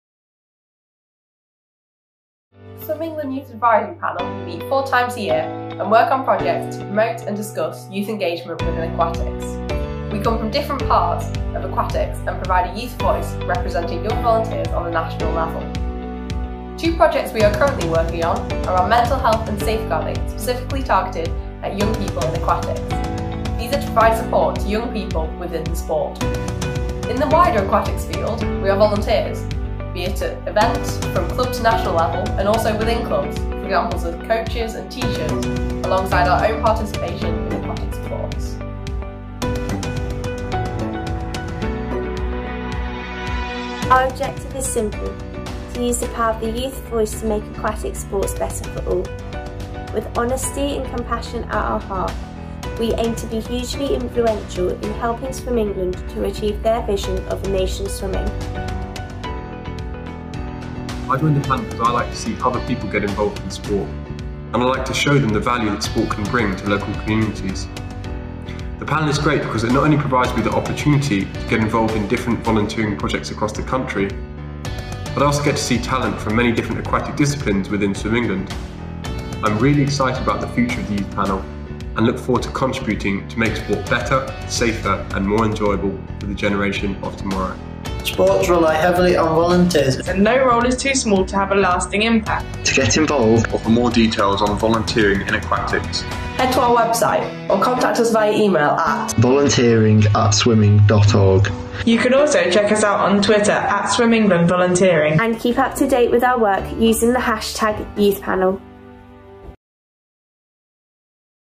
Introducing Swim England's National Youth Advisory Panel
Swim England's National Youth Advisory Panel works on projects to promote and discuss youth engagement within aquatics.